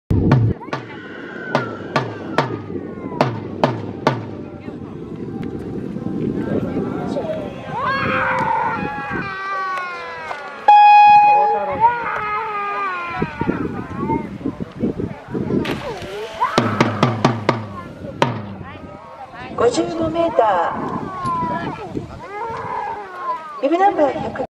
森恢晟 選手 2020イトイカップ ２本目 55メーター 2020/09/19(土) 朝日三望台シャンツェ
札幌福井野中３年時のカイセイ選手。１本目２位で、最終も中学生男子の部２位。